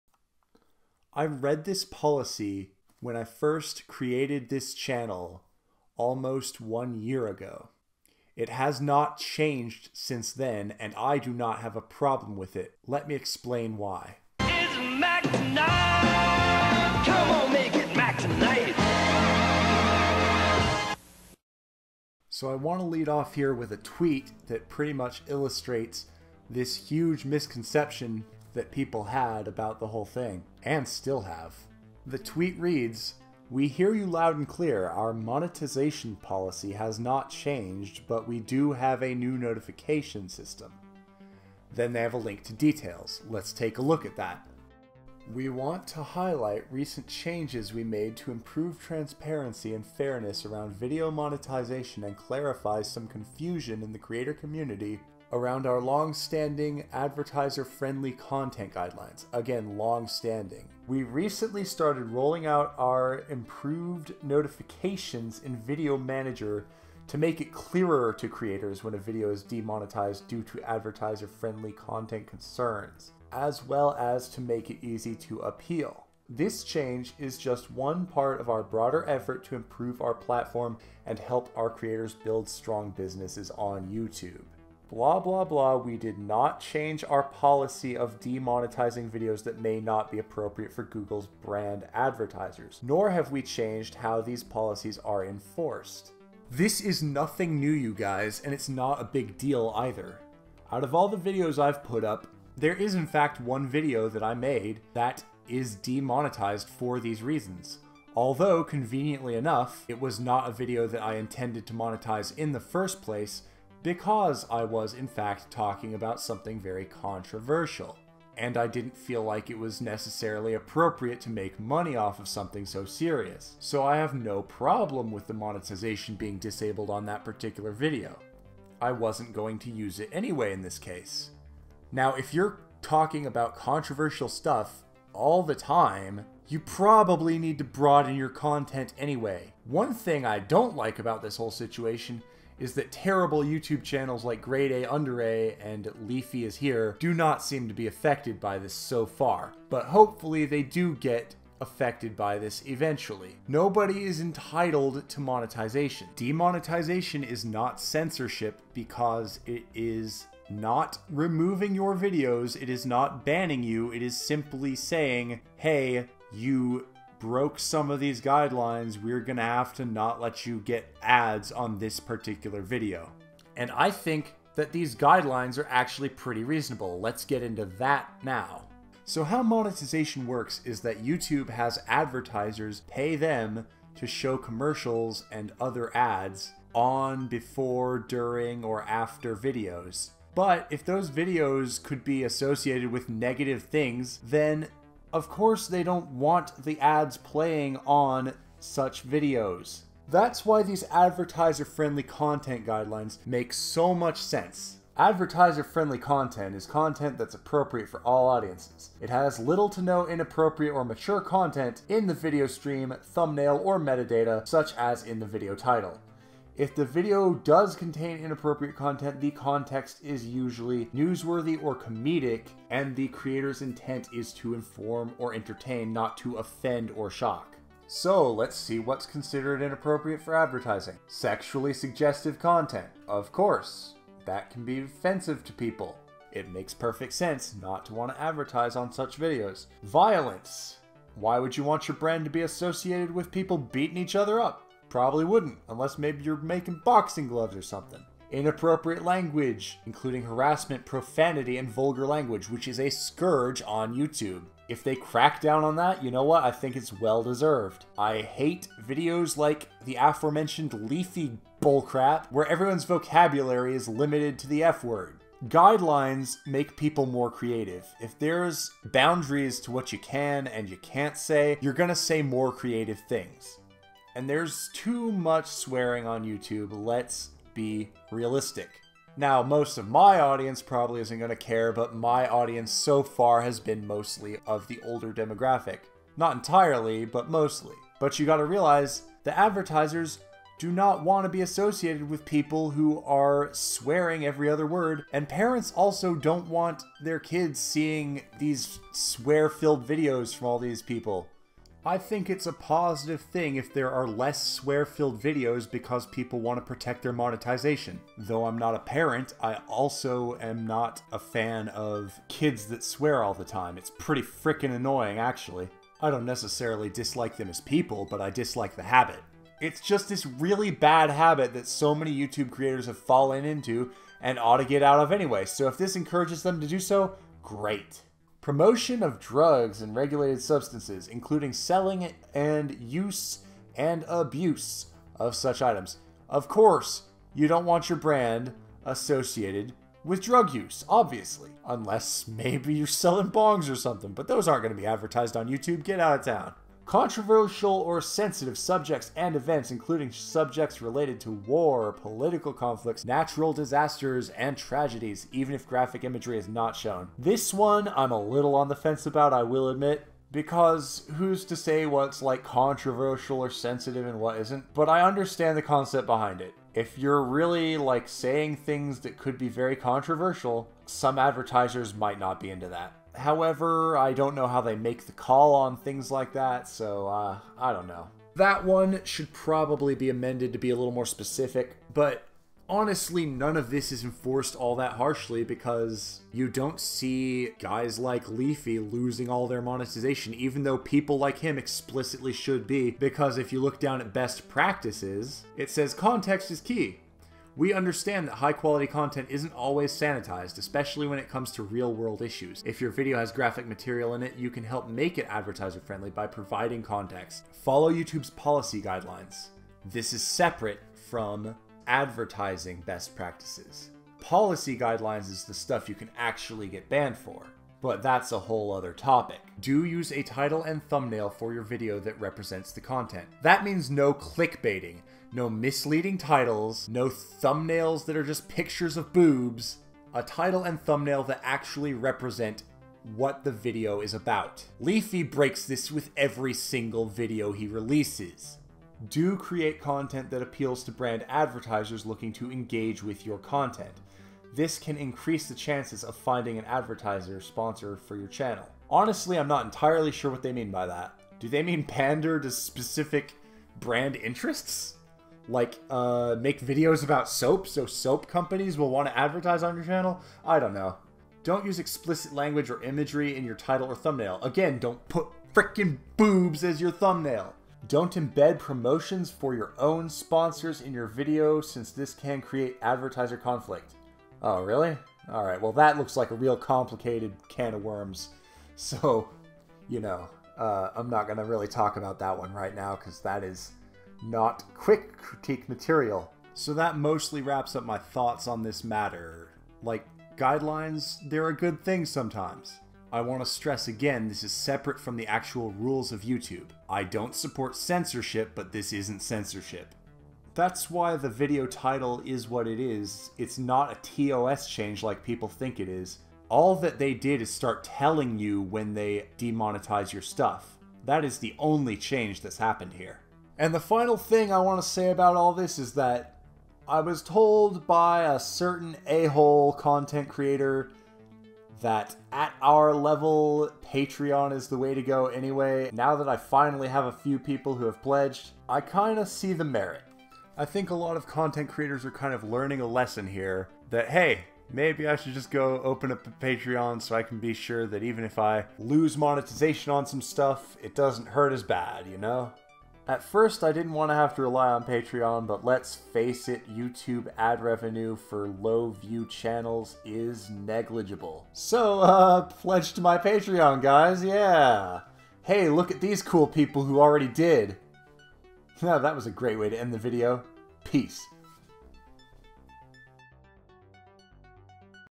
Why I LOVE YouTube's "New ToS"
SPOILER ALERT: It isn't new at all! And all the crybabies should really shut up, because the only real change to YouTube here is that they are now telling creators when they take away a video's monetization instead of doing it silently!